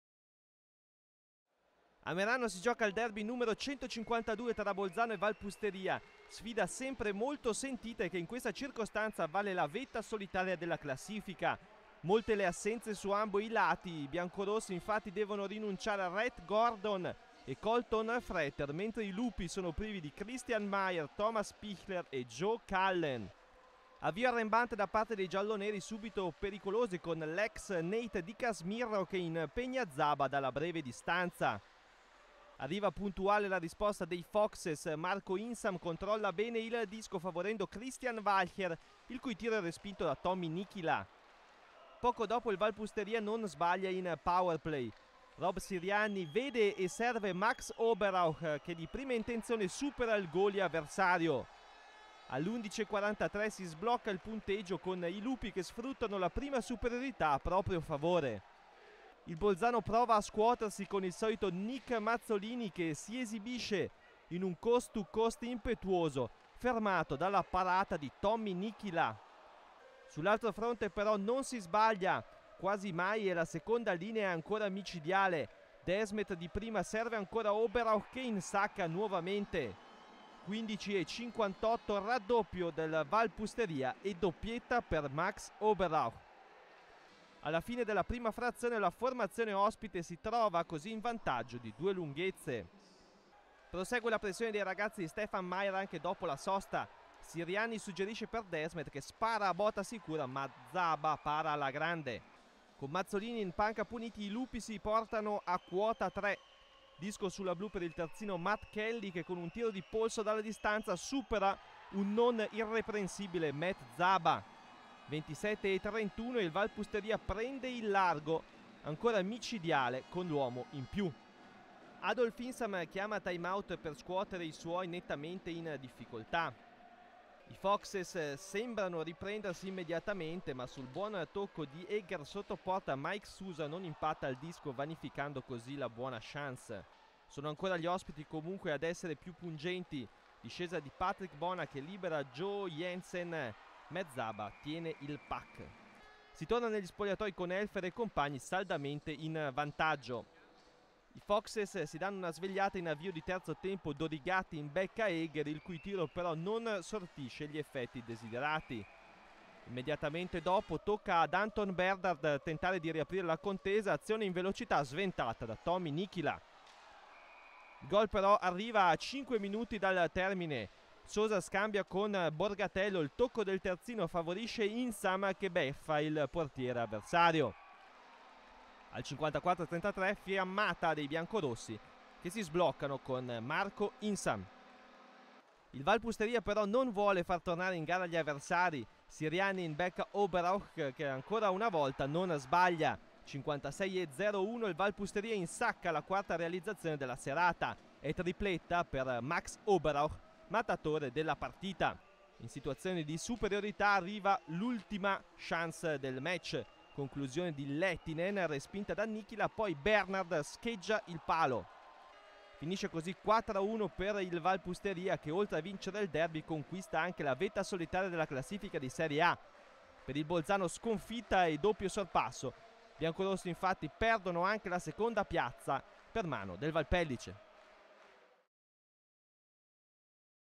Serie A: Bolzano - Val Pusteria
Il 152° derby altoatesino va al Val Pusteria che si impone sul Bolzano con il risultato di 4 a 1